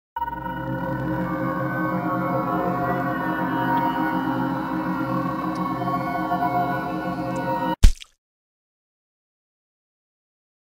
Aprendiendo de efectos (Cabeza explosiva)
Es mi primer intento amateur de utilizar unos programitas que baje para hacer efectos. Nada especial, y tambien ps es un test de mi nueva camara xD